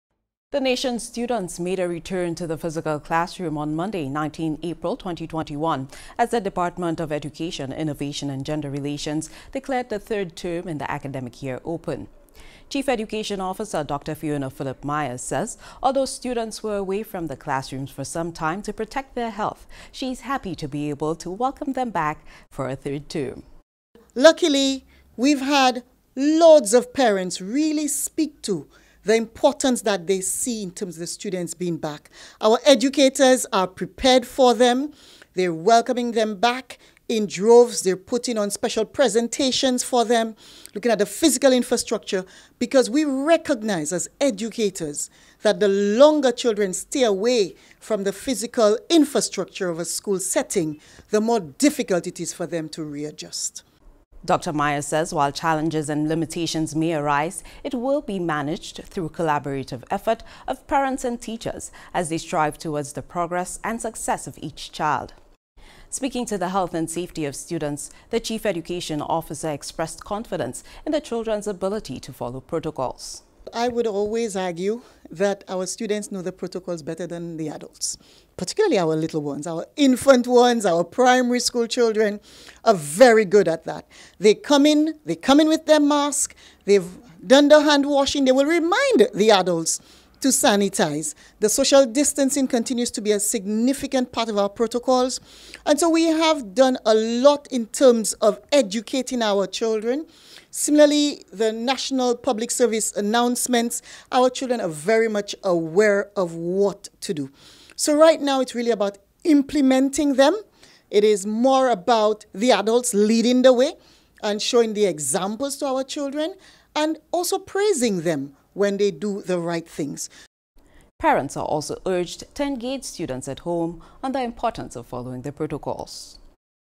Students return to classrooms
The Nation’s students made a return to the physical classroom on Monday, April 19, as the Department of Education declared the third term in the academic year open. Chief Education Officer, Dr. Fiona Philip Mayer, said that although students were away from the classrooms for some time to protect their health, she is happy to be able to welcome them back to school for Term 3.